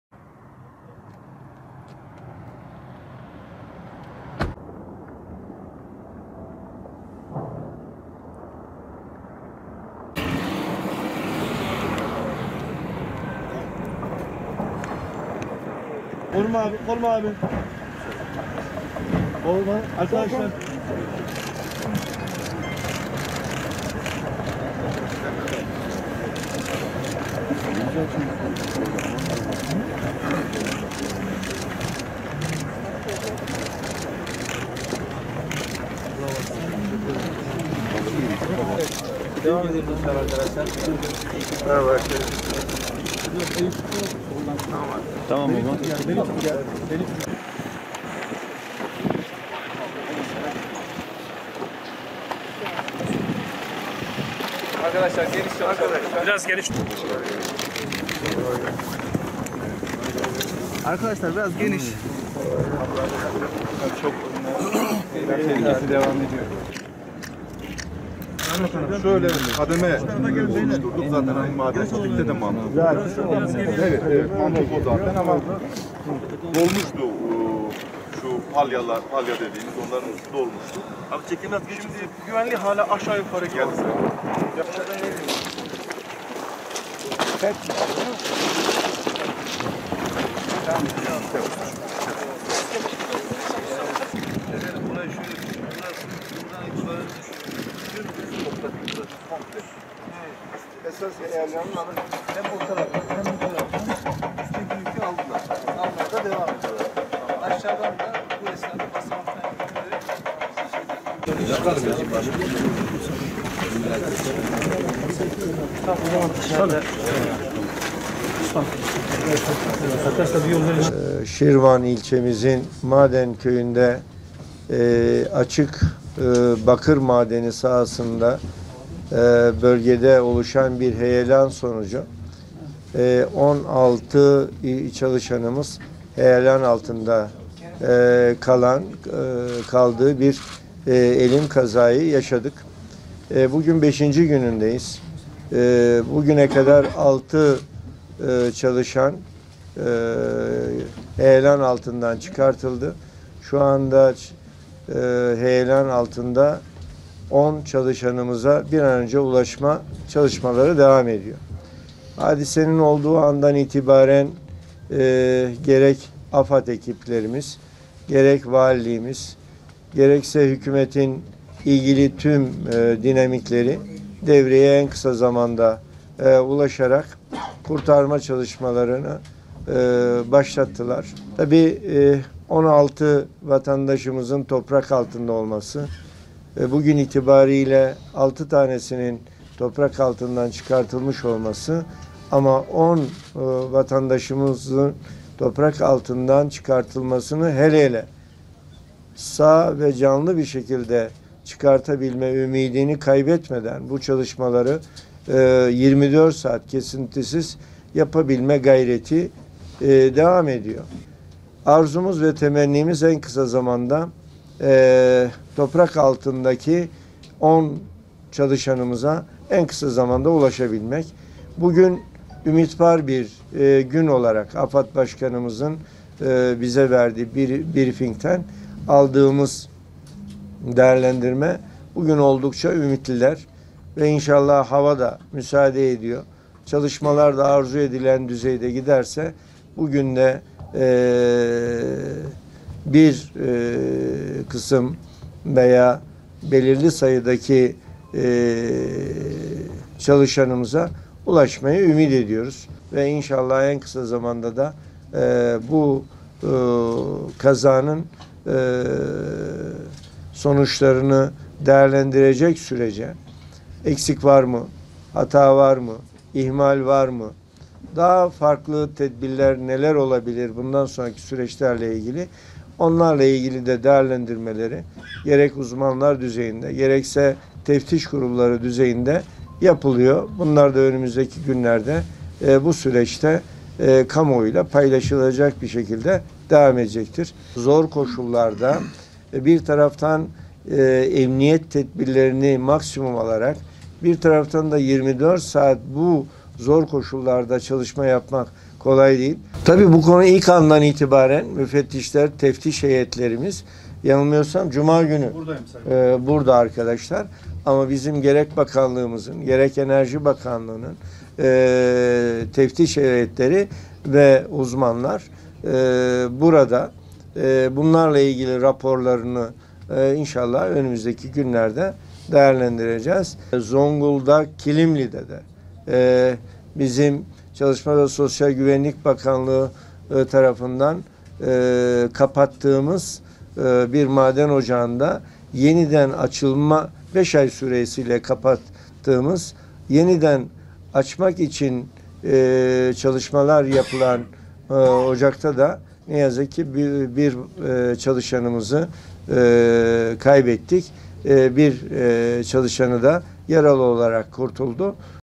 "We hope to reach workers today who trapped in the wreckage"
Minister of Labor and Social Security Mehmet Müezzinoğlu, came to the mining area in Siirt in Şirvan district where the landslide took place, said that if the work goes to the desired level, workers under the dent can be reached today.